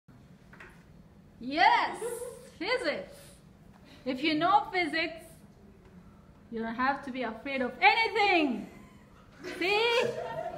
If we apply large enough centripetal force to the rotating bucket, the gravitational force on the water will be balanced and the water will not spill! Physics is Fun!